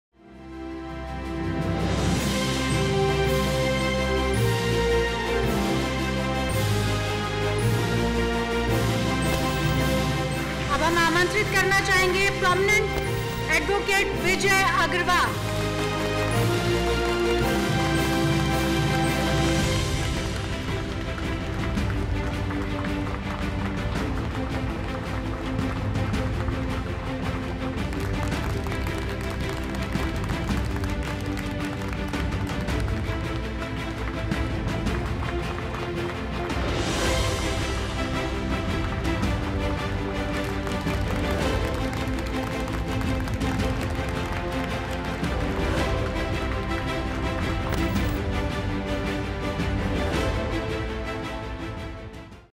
Vijay Agarwal Social Welfare receiving Indian Achievers Award 2019 from Ramesh Pokhriyal
Advocate Vijay Agarwal receiving Indian Achievers Award 2019 from  Minister of Human Resource Development Ramesh Pokhriyal  on 20th January 2020 at Vigyan Bhawan, selected by Former Chief Justice of India K.G. Balakrishnan and Former Supreme Court Judge Gyan Sudha Misra.